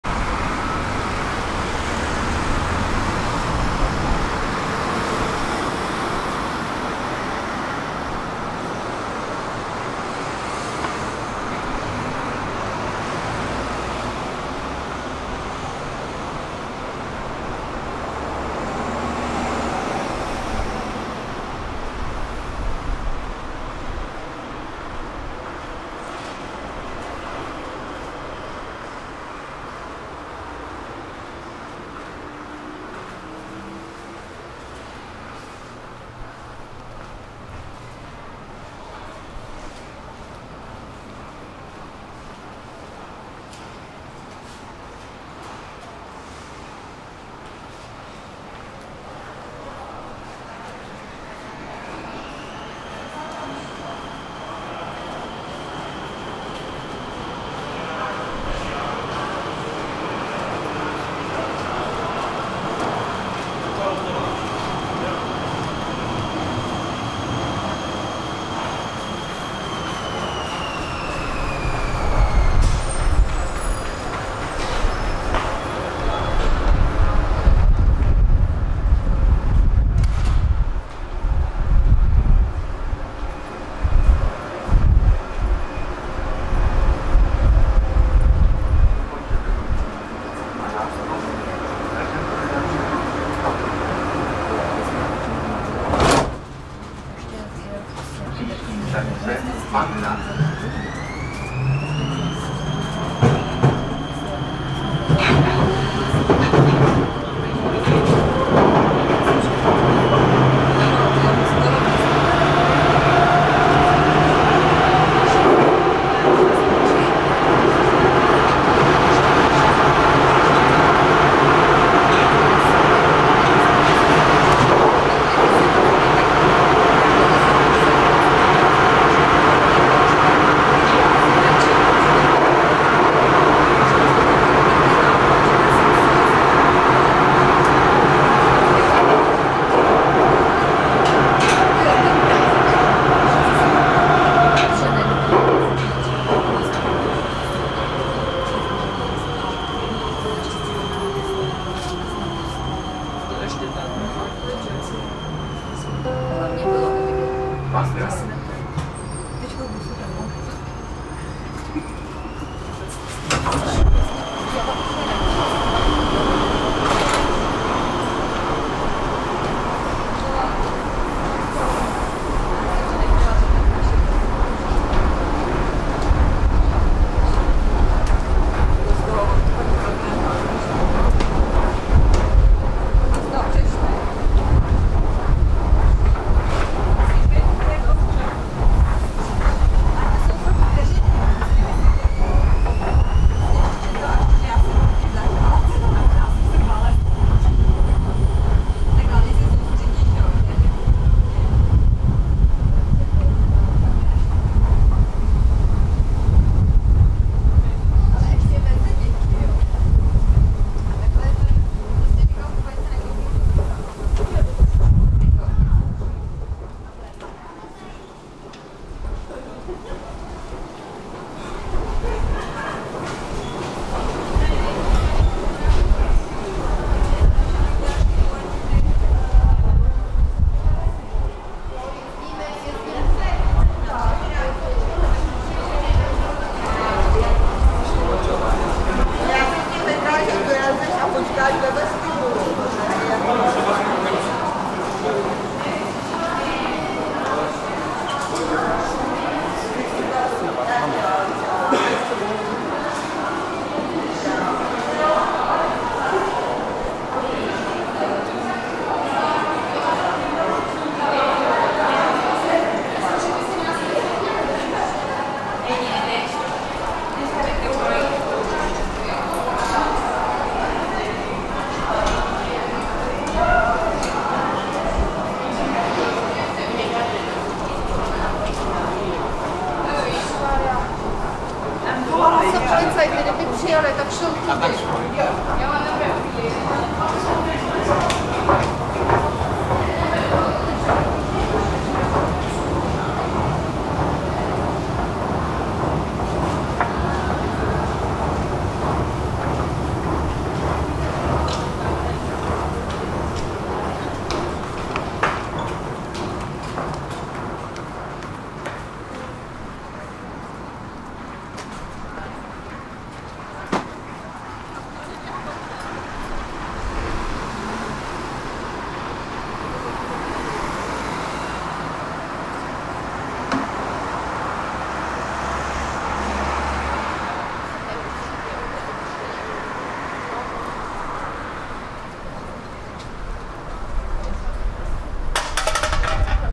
Czech Republic, Prague, metro ride from Pražského povstání to Pankrác
ticket barrier 1:02 , 3:36

metro train 1.18

escalator 3:00 , 4:44

August 2018